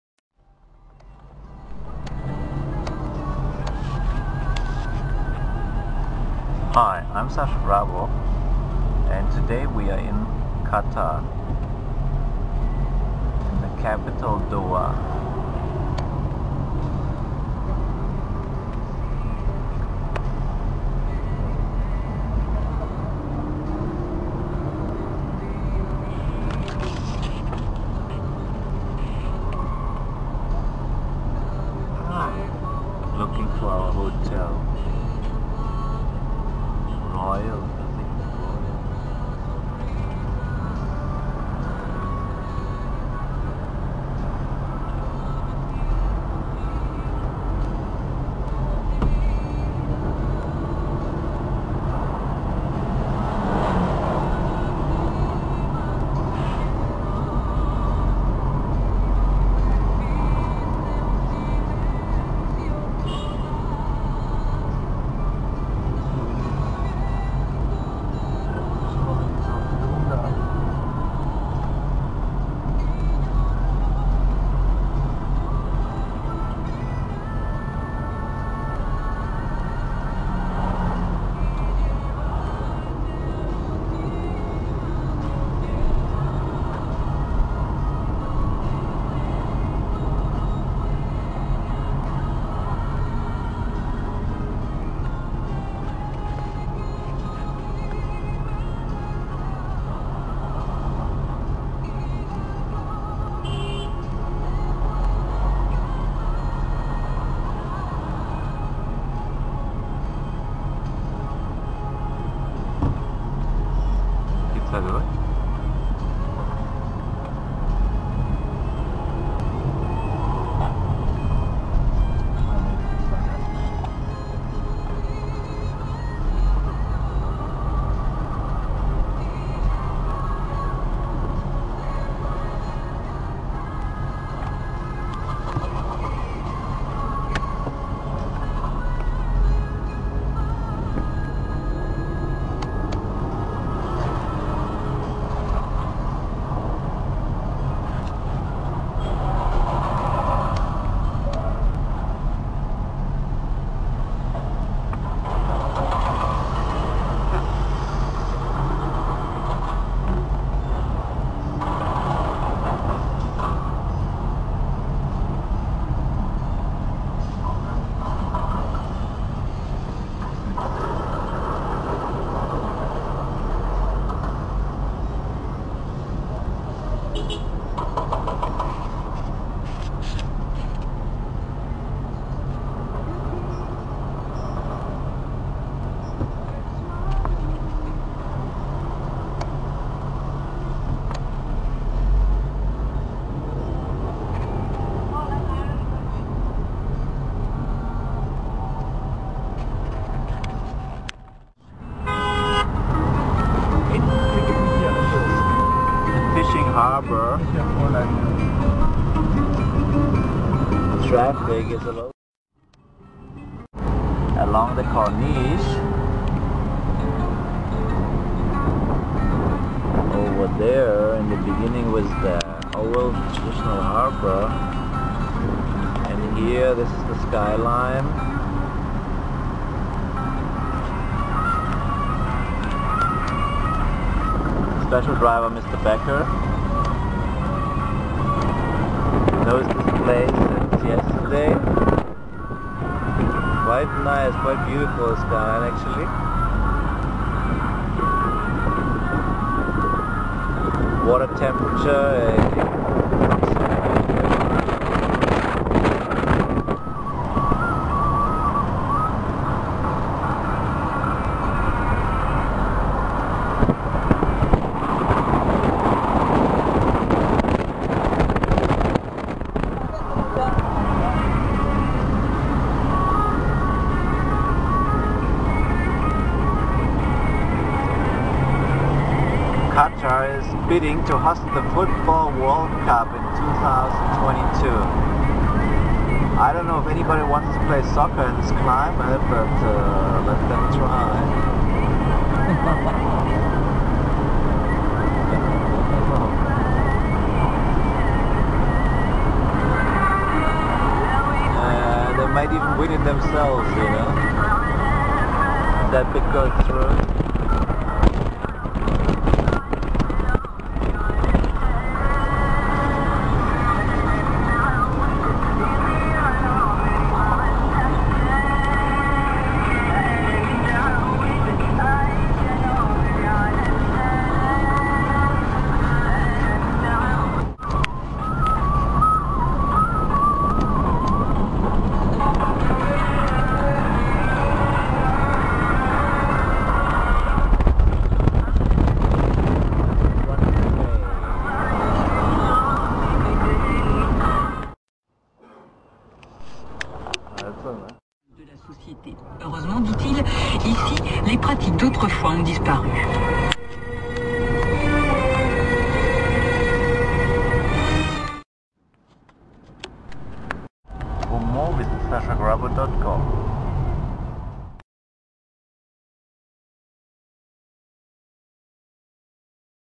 Doha - Qatar الدوحة قطر
Sascha Grabow shows you around Doha, Capital City of Qatar, Middle East. You will get a nice overview, including cityscape, water front, and the desert interior of the country.